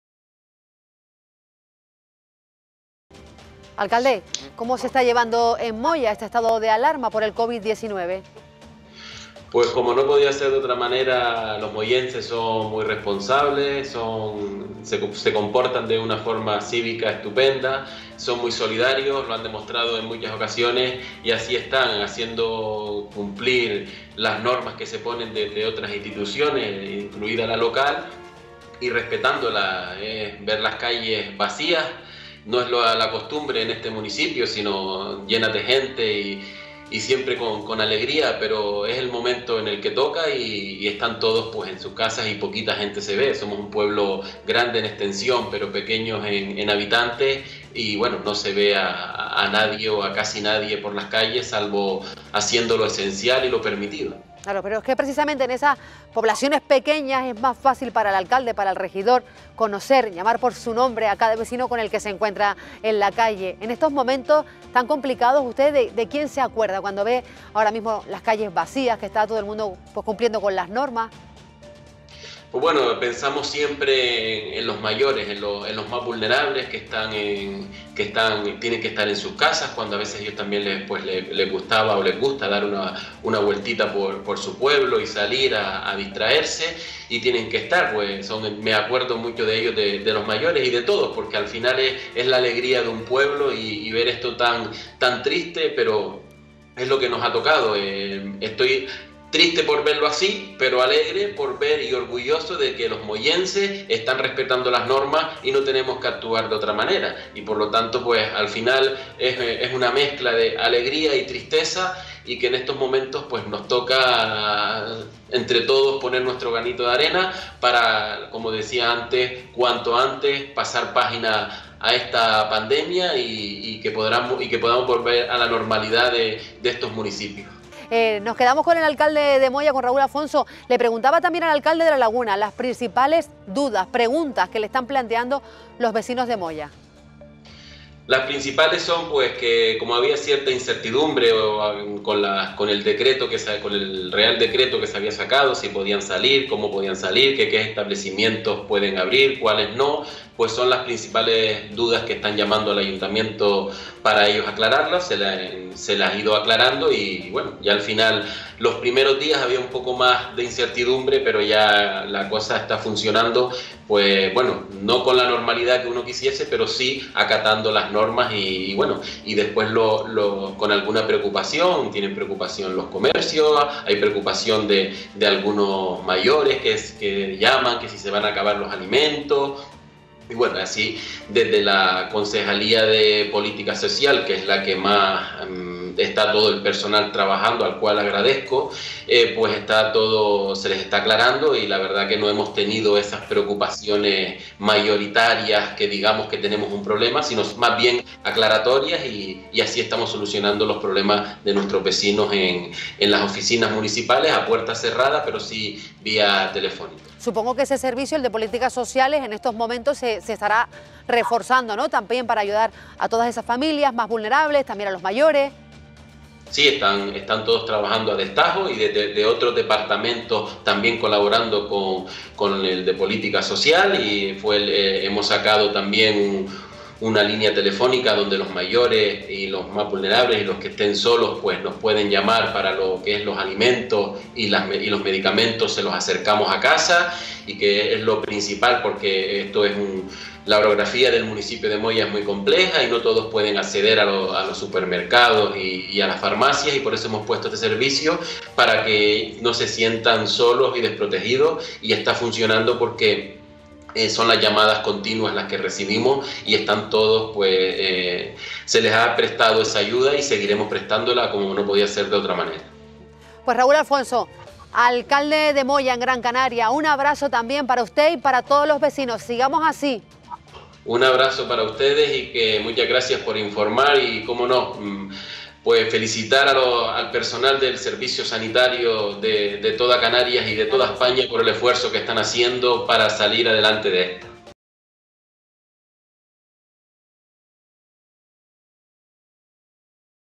Raúl Afonso: "Nos acordamos mucho de los mayores y de las personas más vulnerables"
El alcalde de Moya, en Gran Canaria, asegura que han resuelto las dudas que tenían muchos vecinos, sobre todo, en los primeros días. "Todo el personal de la concejalía de Política Social está trabajando a destajo", afirma Afonso.